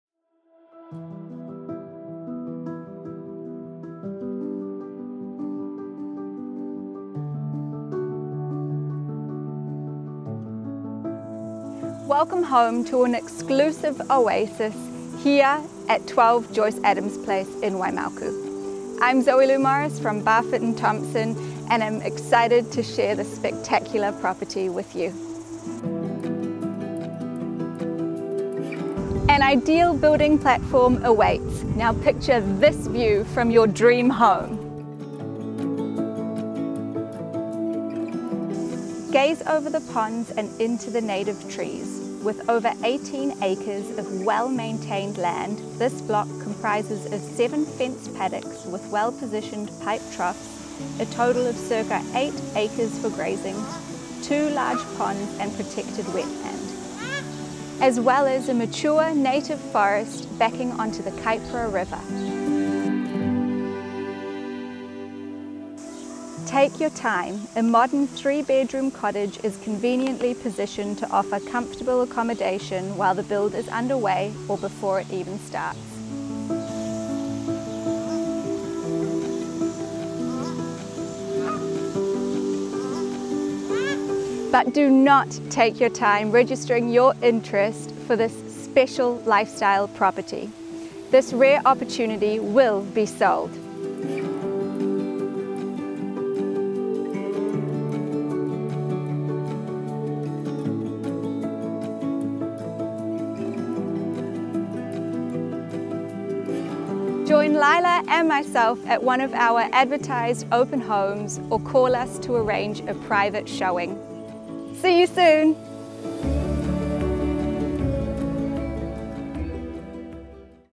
SOLD - 12 Joyce Adams Place, Waimauku - Leila Morris and Zoe-Lou Morris
AUCTION BROUGHT FORWARD: Land, Ponds, Forest - It Has It All

This exclusive 7.5ha (more or less) lifestyle block offers grazing paddocks, picturesque ponds, and mature native forest. Currently running a few cattle, the land could also lend itself to horses ...

Property Marketed by: Leila Morris, West Harbour and Zoe-Lou Morris, West Harbour